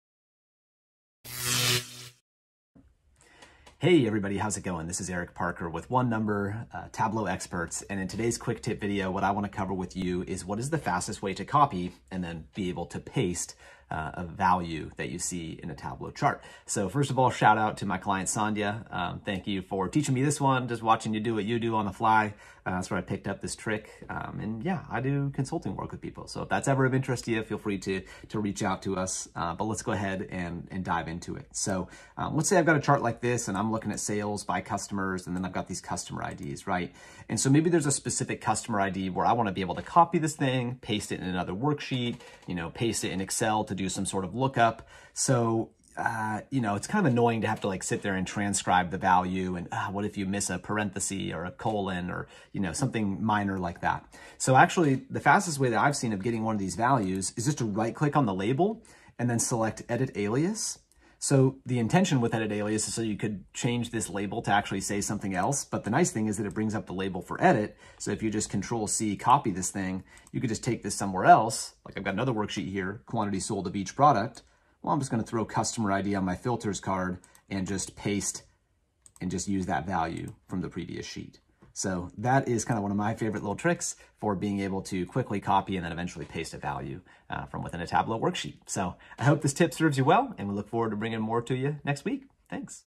Fastest Way To Copy a Value in Tableau (Quick Tip)
Video Description:
Sometimes, you need to copy a value from Tableau to search or reference elsewhere. Previewing the underlying data and copying the value from there is kind of a pain there. Check out this video to learn an easier way to copy a value from a Tableau worksheet!